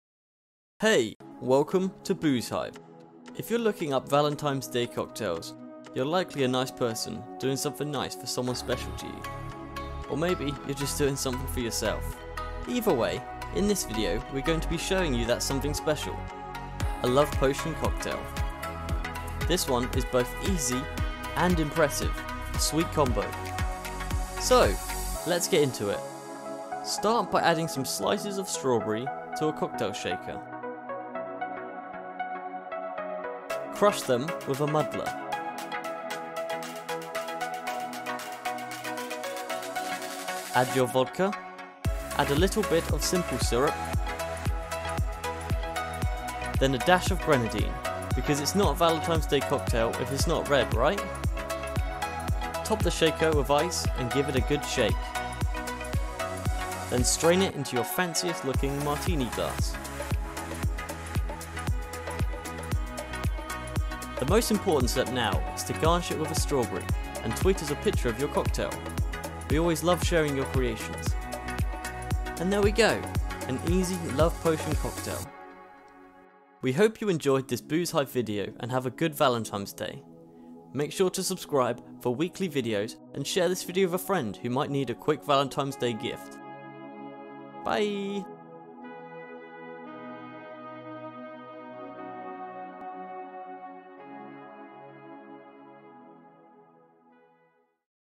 Love Potion Cocktail Recipe! (Valentines Day idea for him/her)
The Love Potion Cocktail, a perfect Valentines day idea for your Boyfriend / Girlfriend. In this Short video, discover how to make the Valentines day cocktail: The Love potion.

Ingredients:
Strawberry Slices
2 Parts Vodka
1 Part Simple Syrup
1/2 Part Grenadine

Chapters:
0:00 Welcome to Boozehive
0:25 What you will need.
0:54 Shake
1:07 Strain
1:25 Outro

We hope you enjoy the recipes!
Our content is always free to watch, however if you would like to show support, feel free to buy us a coffee!☕

Love making drinks?